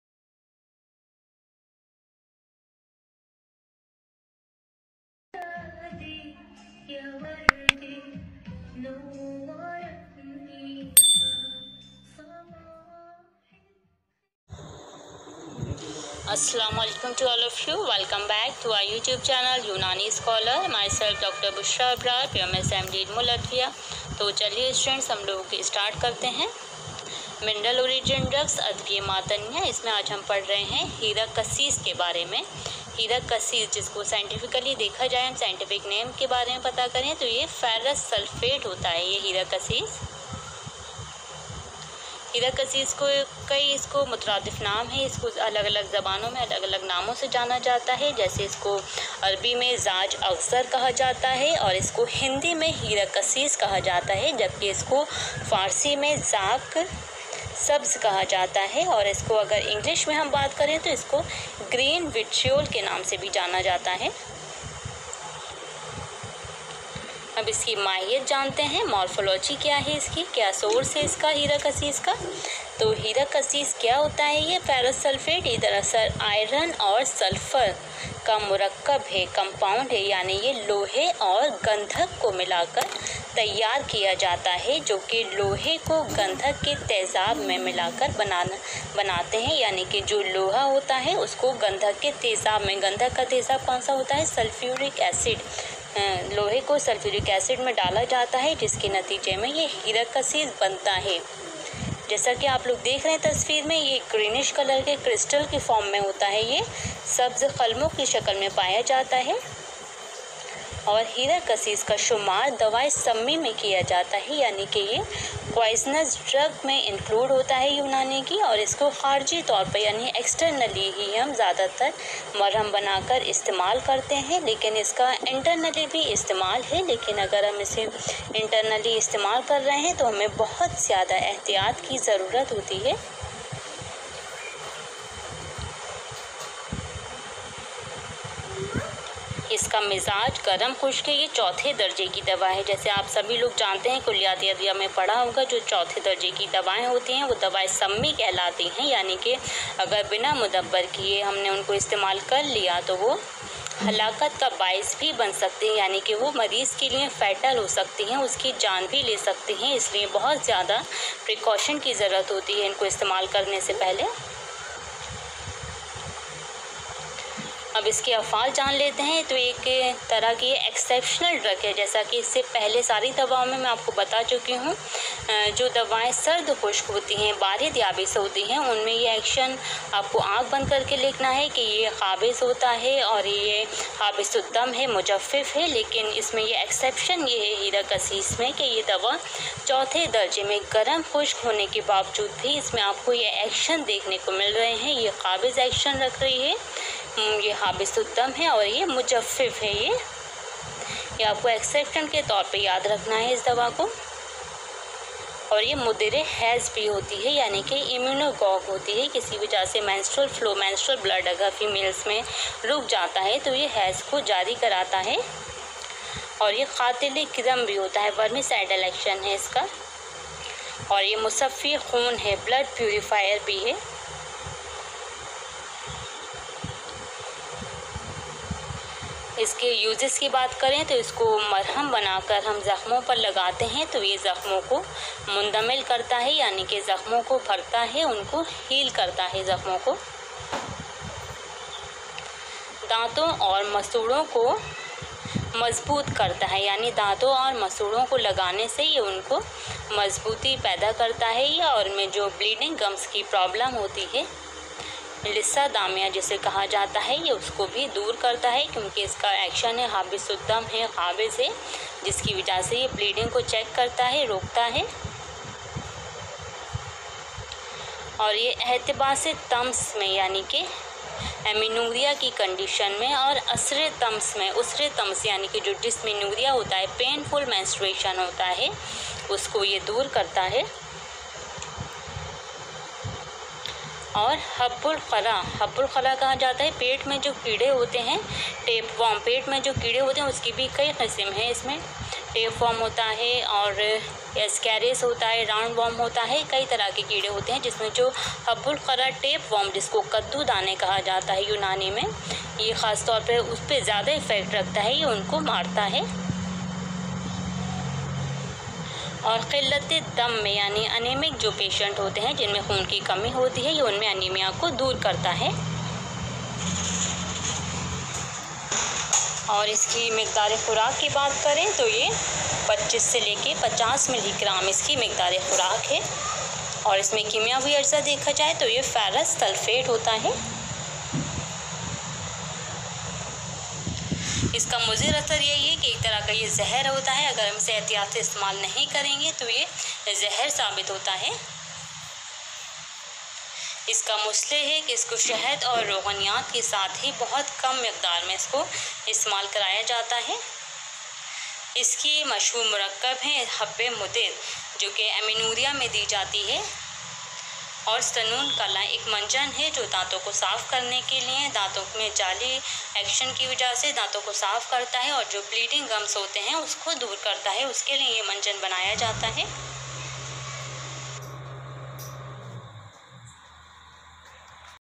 Mineral origin Drugs|| Advia Maadanya||Hirakasis, ھیراکسیس ،Unani medicine||Class lec /Introduction
A comprehensive lecture With slide presentation,Mineral Origin drugs Silver,نقرہ
By :    dr.bushra MD
THANKYOU ❤️!